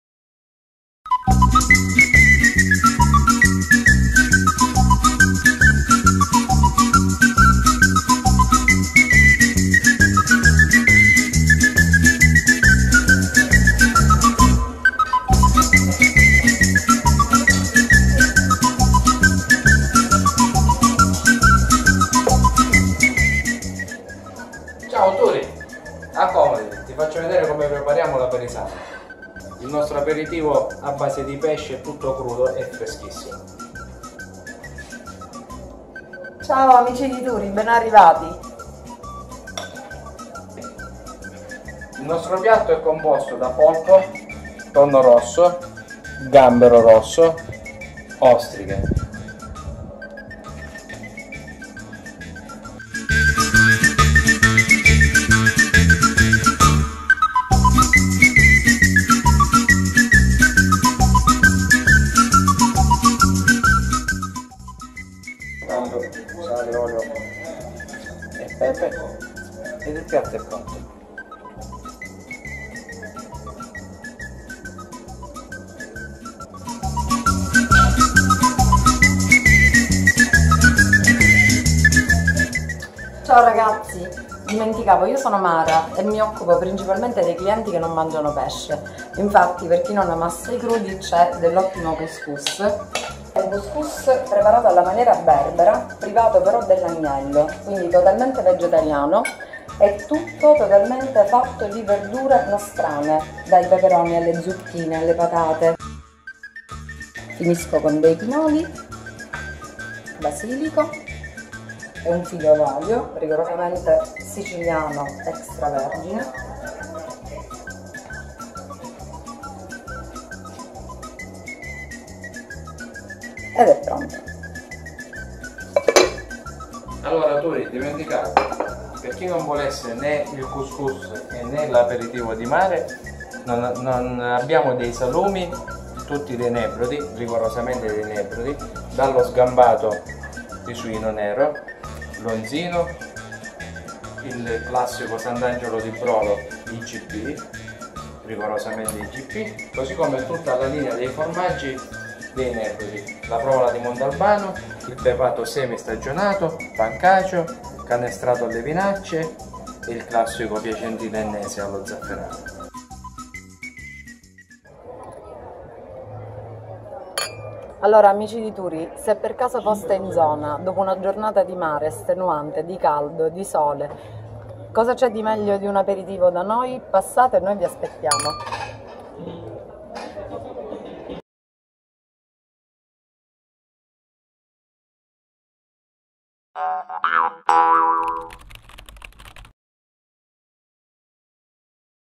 Ristoranti in Sicilia - Sama a Marzamemi per gustare ostriche, gamberoni, cous cous e tanto altro
Aperitivo o cena con pesce fresco e prodotti siciliani Dop a Marzamemi? Tury ha trovato Sama, un piccolo ristorante a 2 passi dal mare in cui gustare cruditè di pesce, cus cus e prodotti tipici siciliani Doc e Dop. Quello che ci voleva per una buona forchetta come Tury...